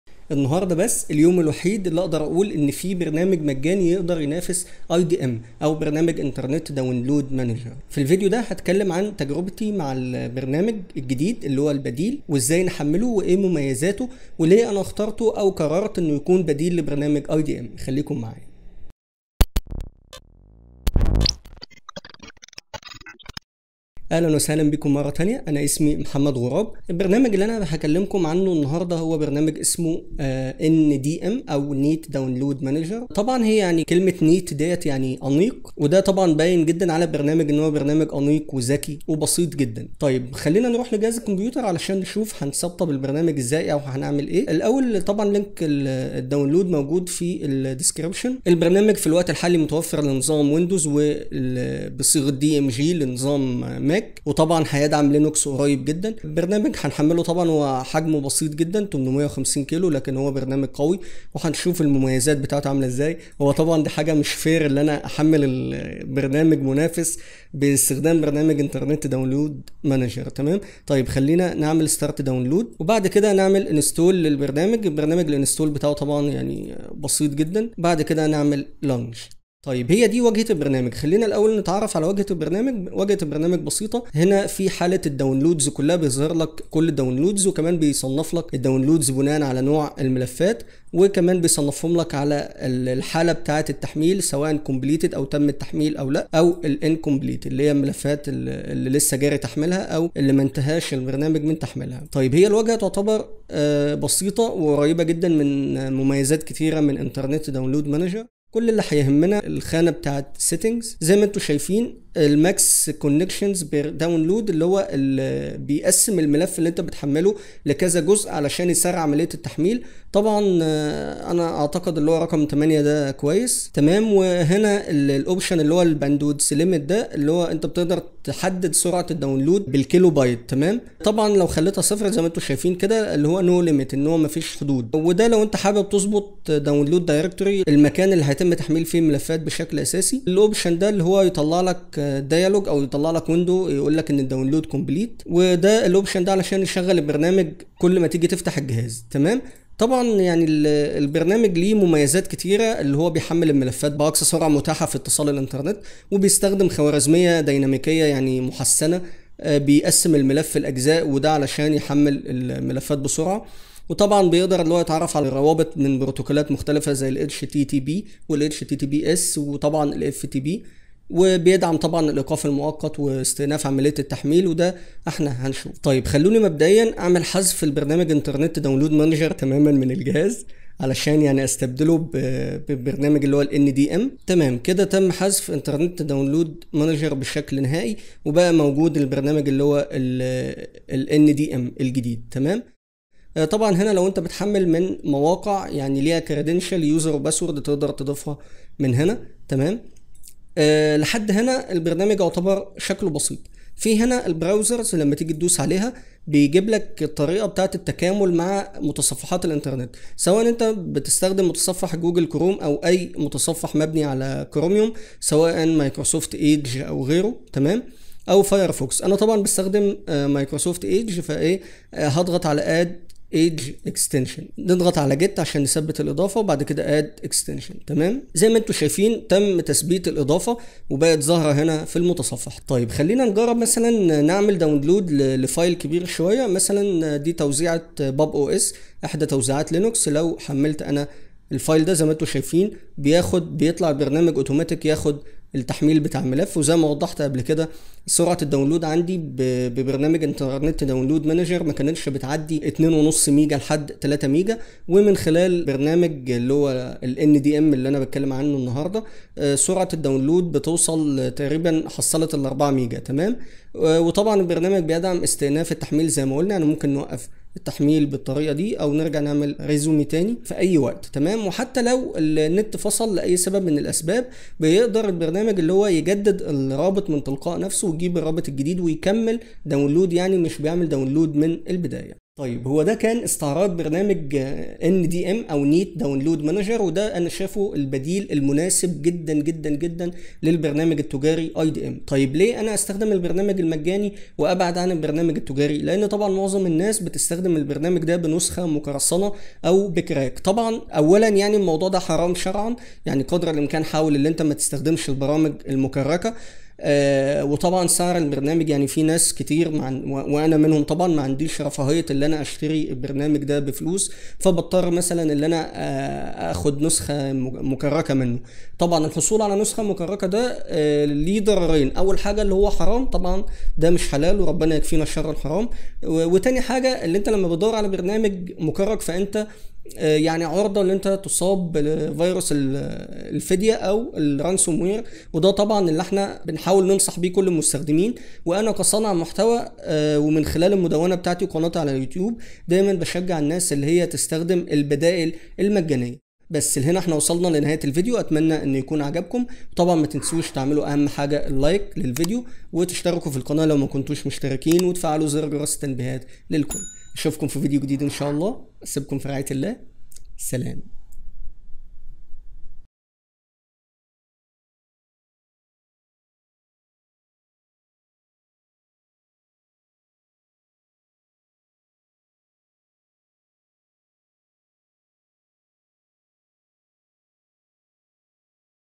أفضل بديل مجاني لبرنامج التحميل IDM بلا منازع Neat Download Manager
Neat Download Manager [NDM] برنامج لتحميل الملفات من الانترنت بسيط وسهل الاستخدام، ويعتبر بديلاً مناسباً لبرنامج IDM، لذلك قُل وداعاً للتعرض للمواد القرصنة والإصابة بفيروس الفدية.

00:00 مقدمة الفيديو
00:26 تحميل البرنامج
01:19 التعرف علي واجهة البرنامج
01:48 مميزات البرنامج
03:12 تجربة استخدام البرنامج
06:02 لماذا برنامج NDM
07:06 نهاية الفيديو